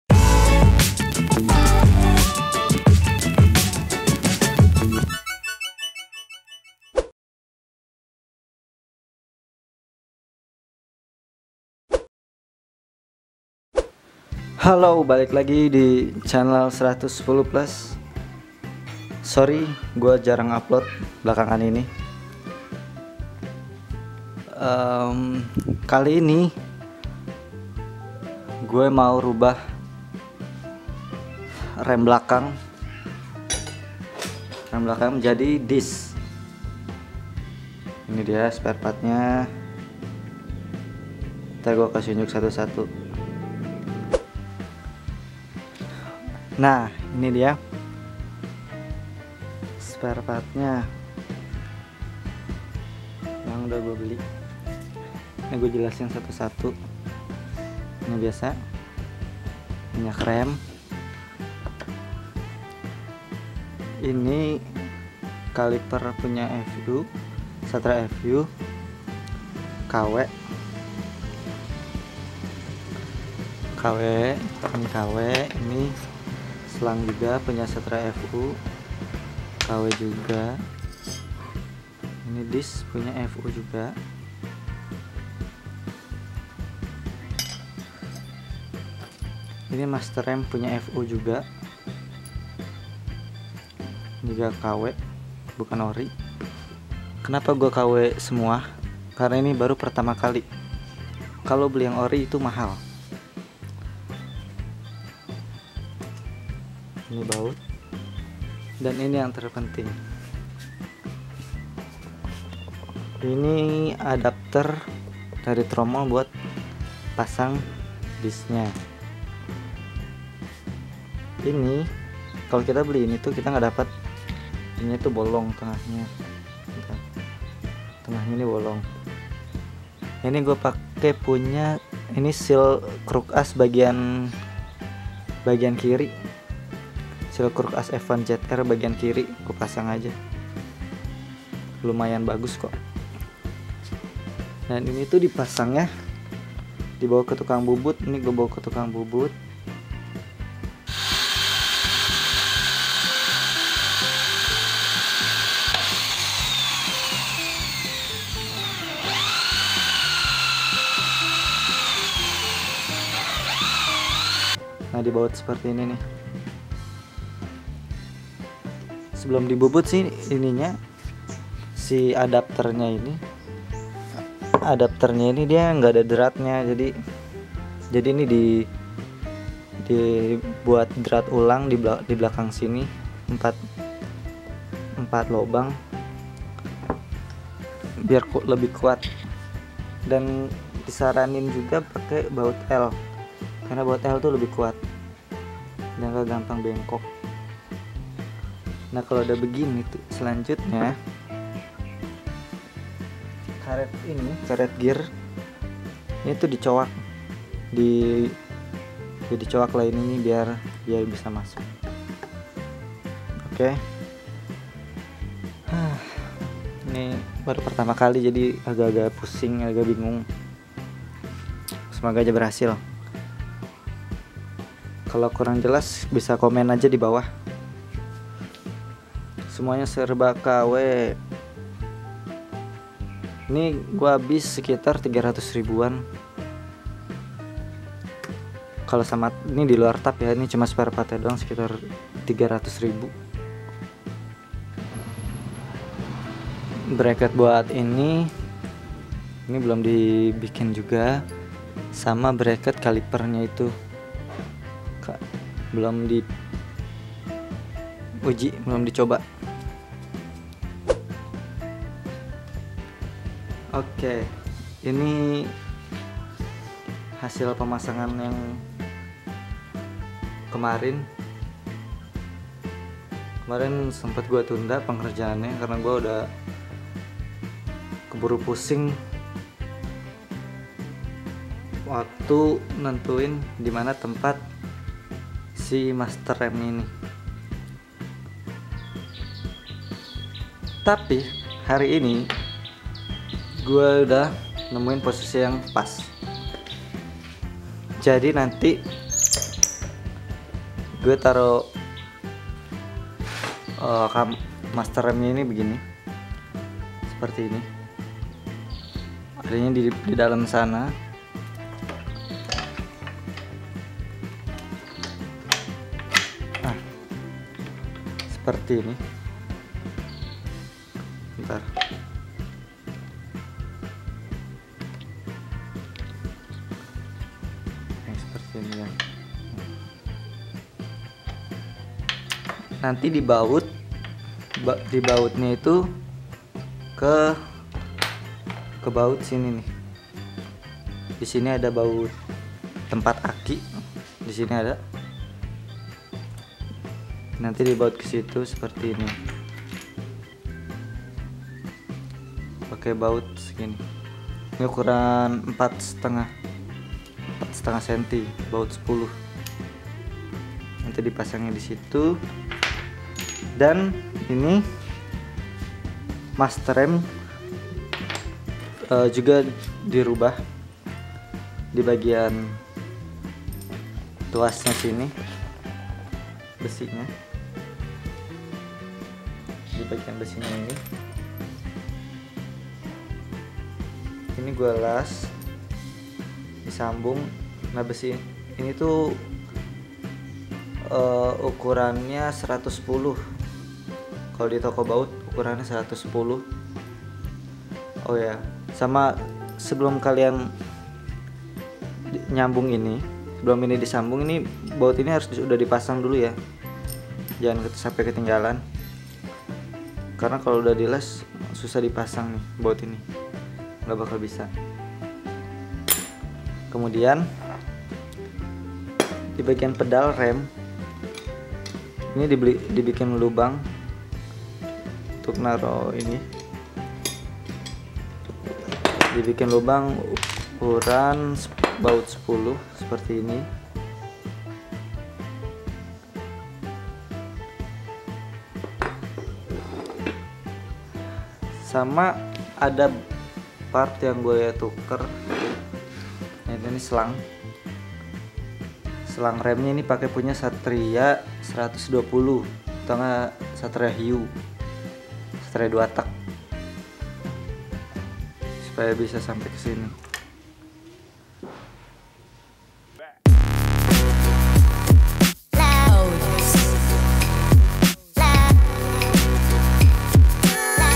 Persiapan Upgrade rem F1ZR - PART 1 [Tutorial]
Proses pemasangan rem disc belakang F1ZR ini saya bikin jadi beberapa part karna durasi yg panjang...